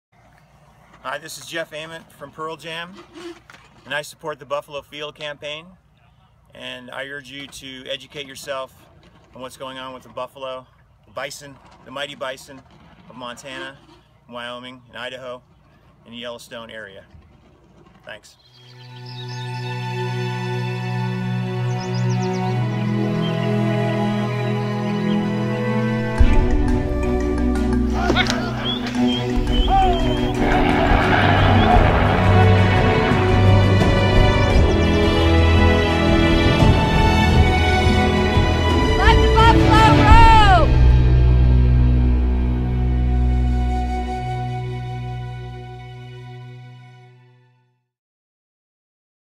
Jeff Ament, bassist for Pearl Jam, supports Buffalo Field Campaign
Jeff Ament, bassist for Pearl Jam and Buffalo Field Campaign advisory board member, wants you to support Buffalo Field Campaign.

Threatened by government agencies to appease livestock interests, America's only continuously wild buffalo population is in dire need of protection. Buffalo Field Campaign is in the field with the buffalo along the Yellowstone boundary, documenting and protecting this iconic species.